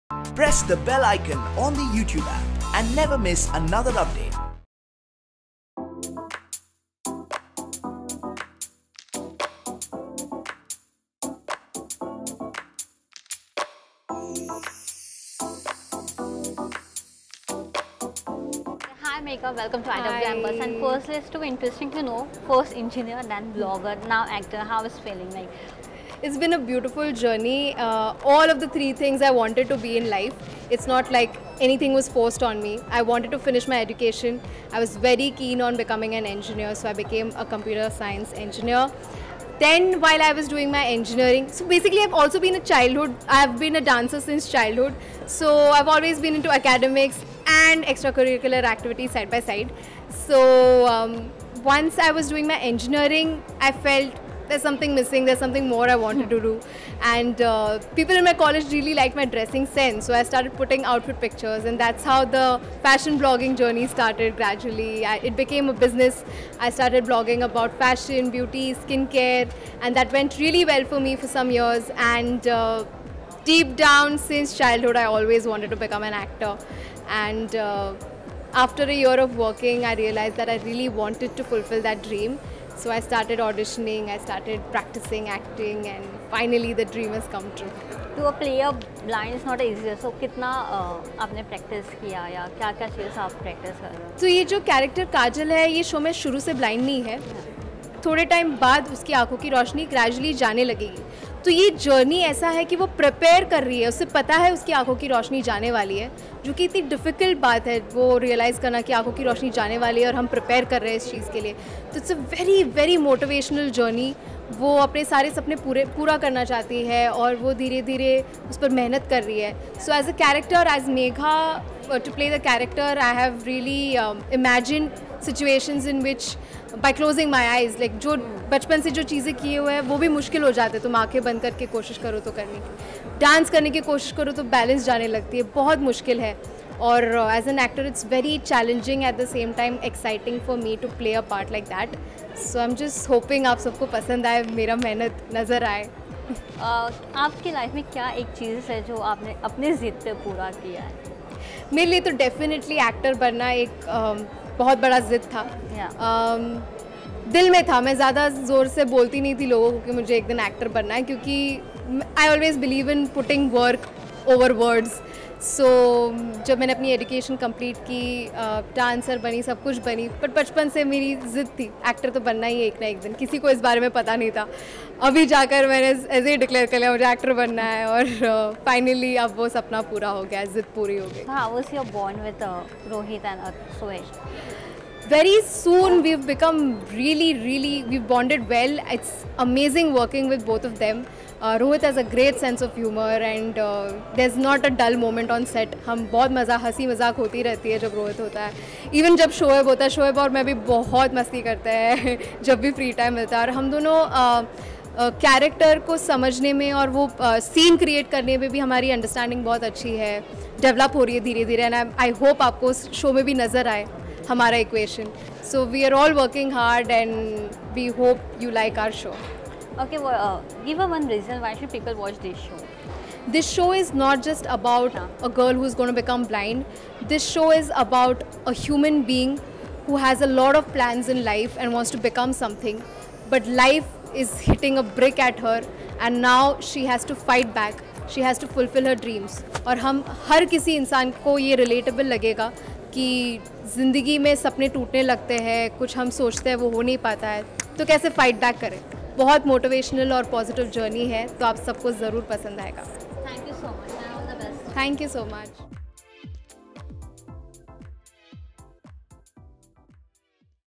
Megha Ray says 'To become an actor was a big achievement for me'
MeghaRay talks on her debut show

/apps/details?id=com.iwmbuzz.indianwikimedia